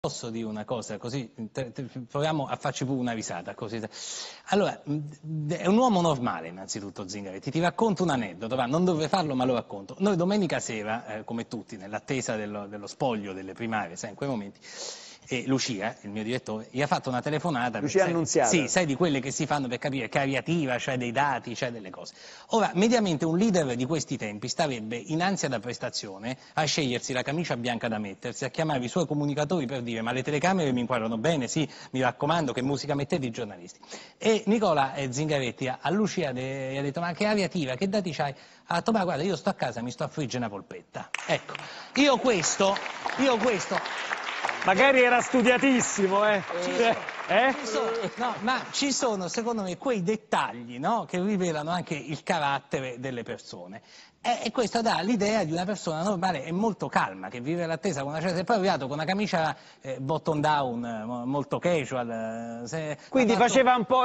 De Angelis: ‘La sera delle primarie Zingaretti era a casa a friggere polpette’
Alessandro De Angelis (Huffington Post): Vi racconto un aneddoto su Zingaretti, la sera delle primarie era molto molto calmo…’